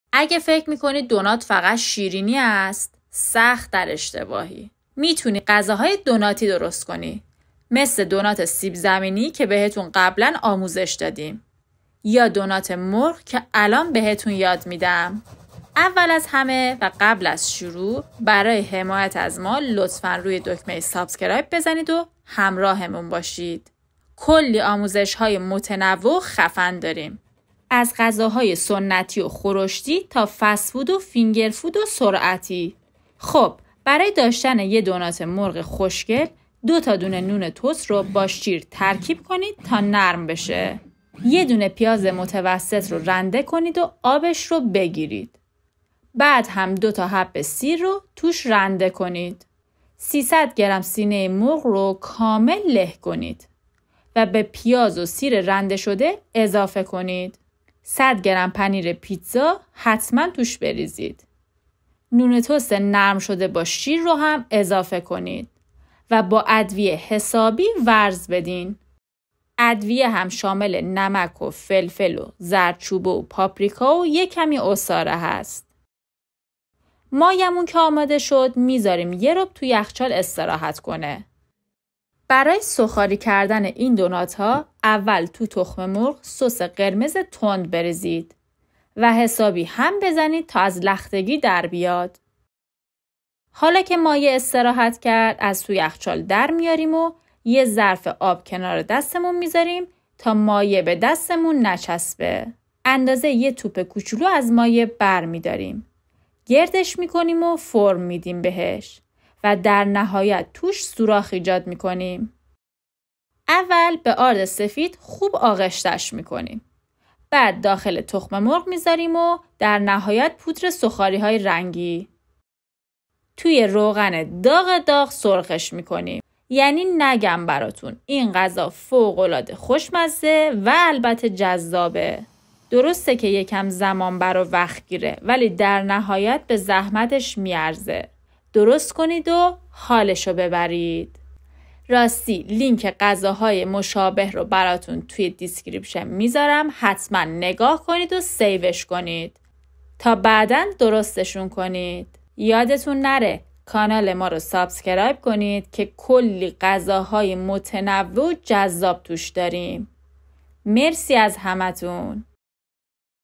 آموزش دونات خوشمزه سوخاری 🐔 طرز تهیه دونات مرغ کریسپی
آموزش  دونات مرغ 🐔

سلام به همگی 🌼🌻

  میدونستی با مرغ خیلی غذاهای متفاوت میشه درست کرد؟ توی این ویدیو آموزش یه غذای ساده رو میبینی  که مواد اصلیش که مرغ و پنیرپیتزاست در دسترس همه هست 🥯 اگه این دونات مرغ لذیذ وترد رو با آموزش من درست کردی حتما کامنت بذار و نظرتو بهم بگو🌺ممنون که با لایک و کامنت من رو حمایت میکنی عزیزم🌷

مواد لازم برای طرز تهیه :

سینه مرغ 300 گ
پیاز 1 عدد
پنیر پیتزا 100 گ
ادویه

آرد سفید
پودر سوخاری